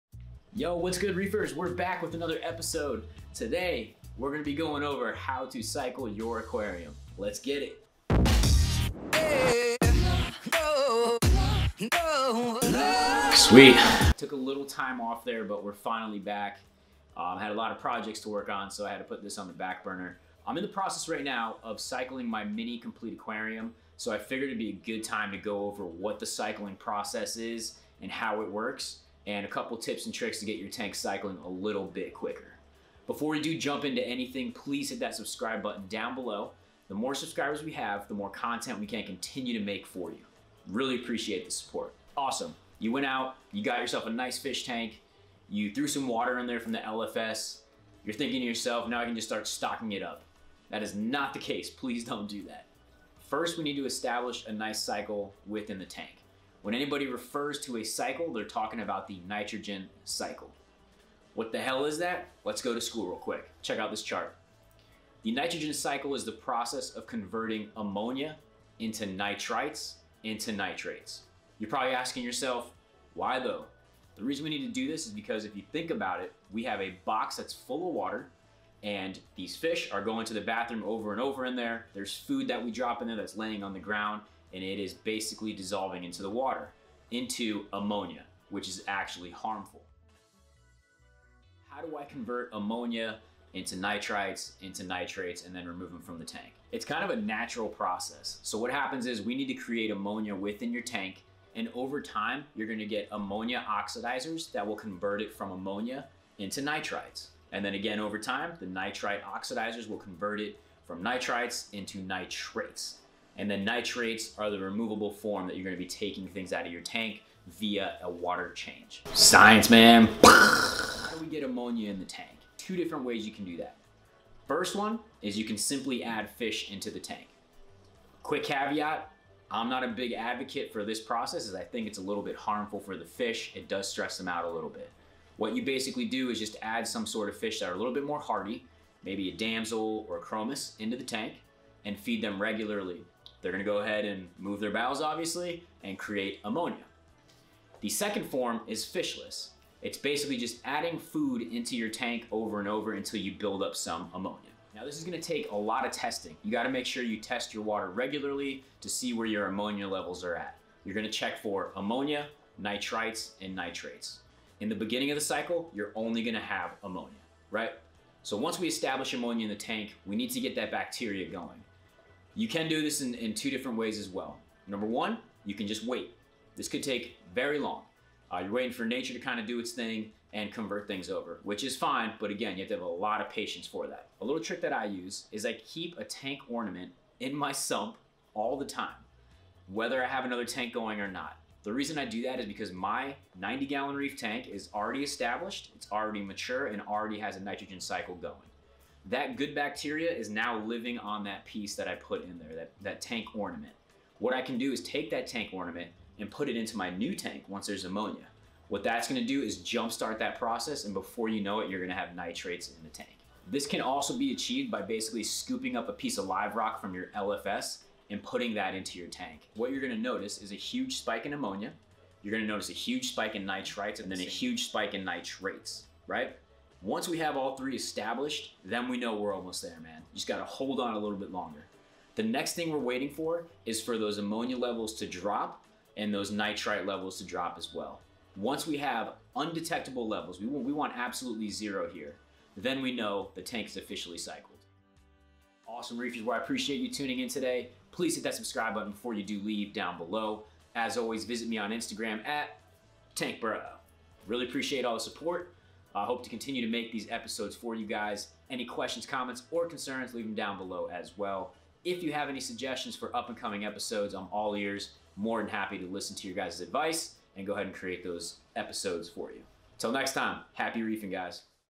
Beginners Guide to cycling your new fish tank
Hey Reefers! We are back! This episode walks you through what cycling is, why we need to cycle, and how to cycle your tank!

If you have any suggestions for up and coming episode let me know as well!

Happy Reefing!